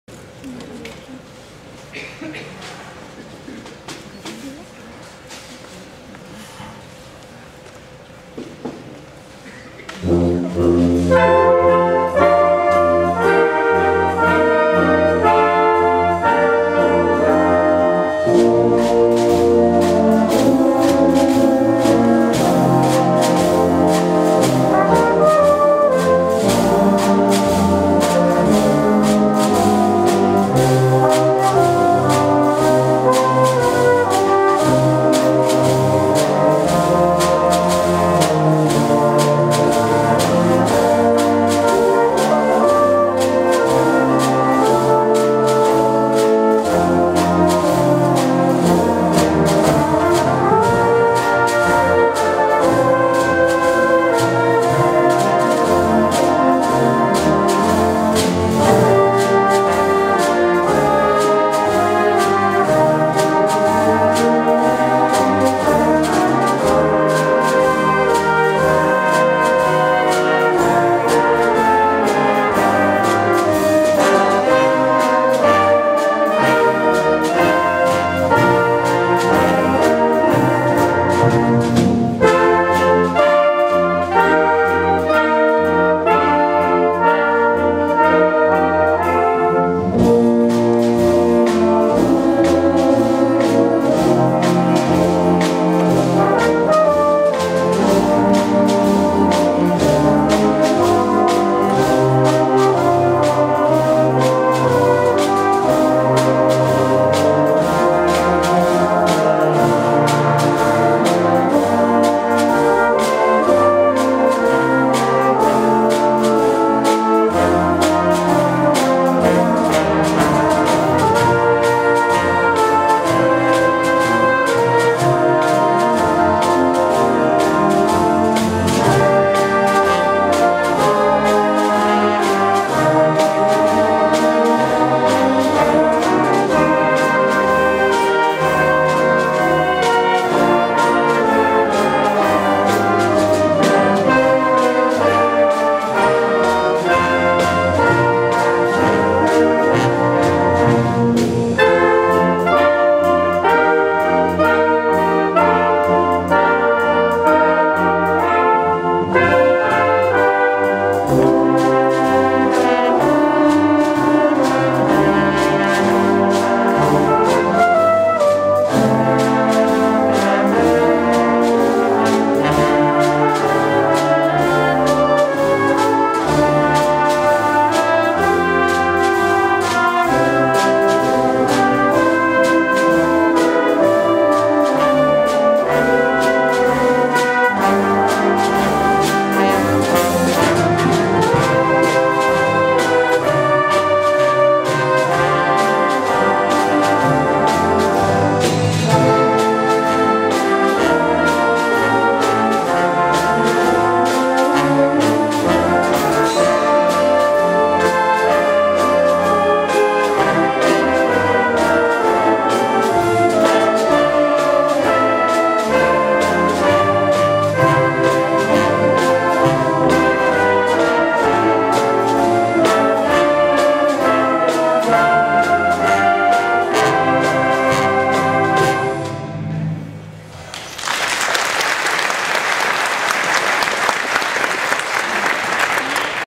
KT-W bij Kerstvolkszang Zuidland - 22 december 2012
Medewerking aan kerstvolkszang door De Kleine Trompetter / Wilhelmina op 22 december 2012.